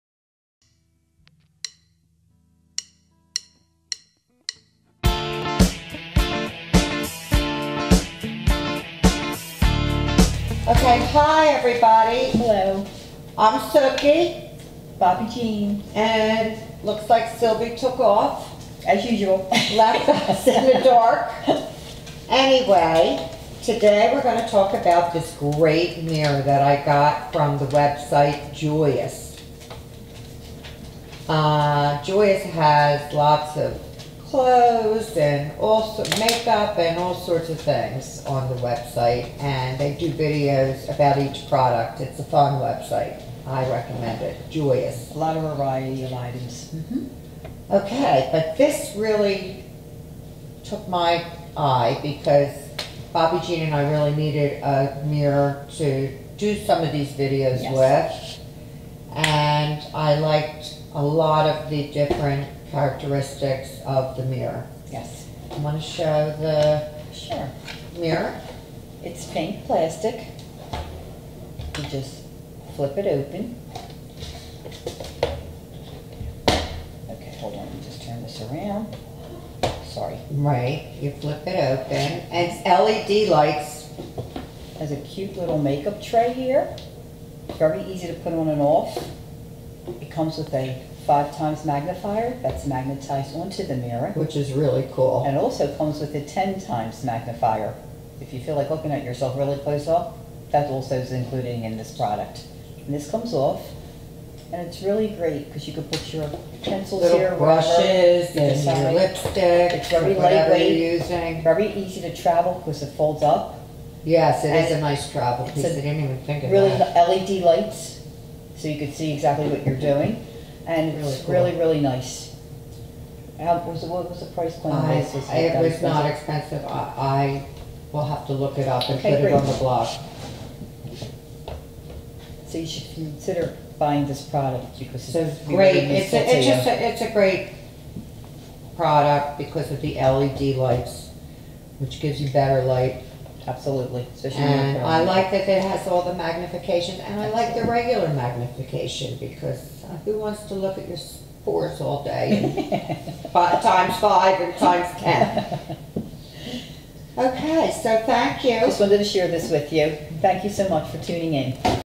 JOI SPOTLITE HD PROFESSIONAL MAKEUP MIRROR FOR PERFECT MAKEUP - Review
This mirror gives you the perfect lighting you need for applying your makeup perfectly.  The premium LED lights produces ultra bright natural daylight.  You get magnifications 5X and 10X, it tilts and rotates, and it is portable.